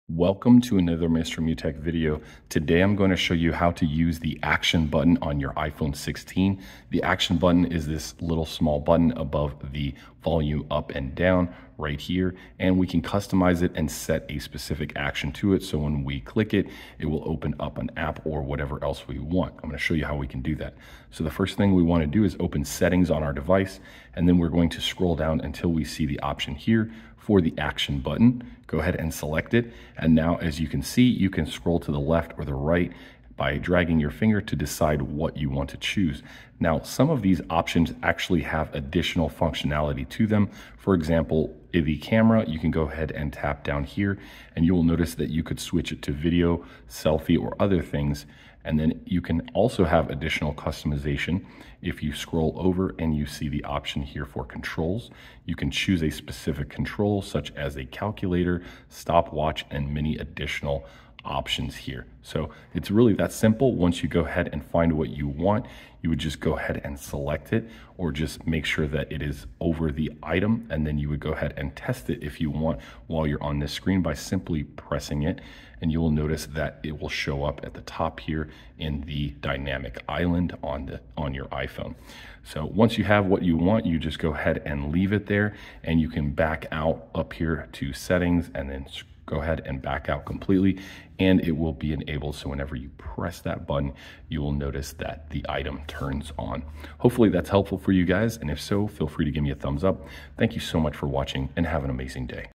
How To Use Action Button On iPhone 16! (Quick Guide)
This quick guide shows you how to use the action button on iPhone 16 and customize it within settings.

Note: The device used in this video is iPhone 16 Pro Max model with iOS 18 installed.